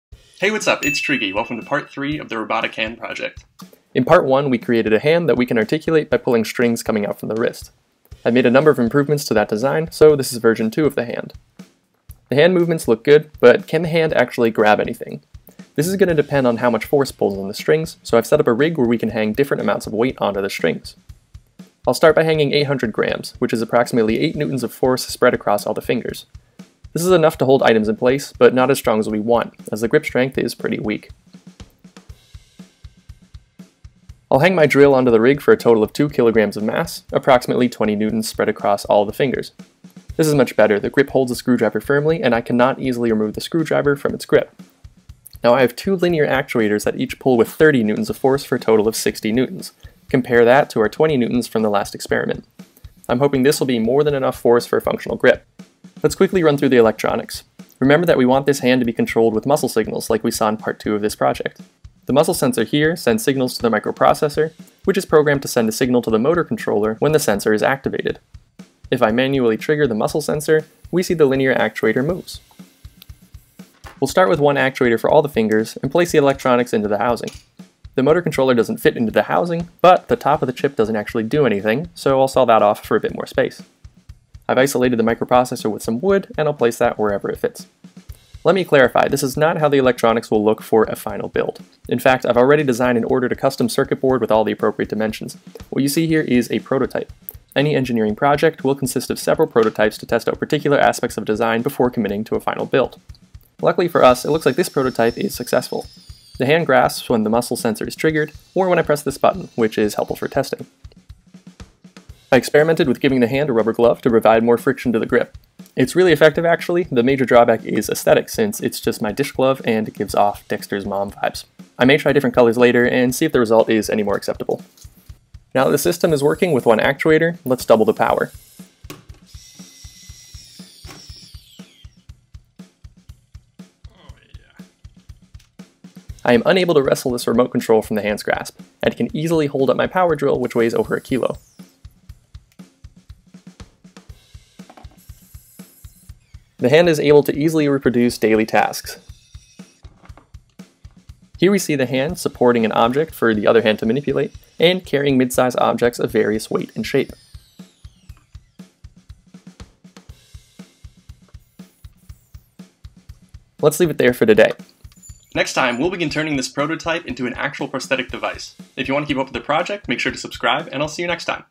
3D printed robot hand prosthesis | Part 3
Today we'll put together a first complete prototype of the hand prosthesis using tools from parts 1 and 2.